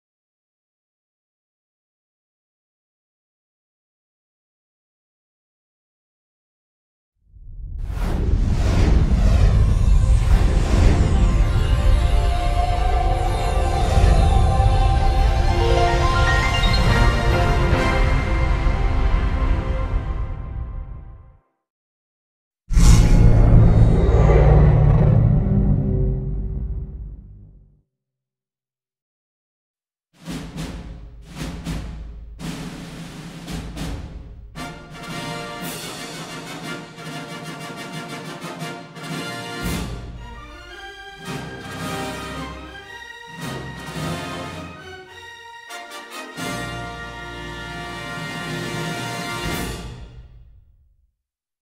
CBS Home Entertainment / Source Filmmaker / 20th Century Fox (2019) (HD, 720p) (L4D2 Project) (FREE)
I will making of Back from the Undead An Left 4 Dead 2 Movie Project on December

I made 20th Century Fox Logo little Dark and Filter so I can use it for my SFM movie

Feel free to use for a Full Movie of Left 4 Dead 2 Movie SFM Project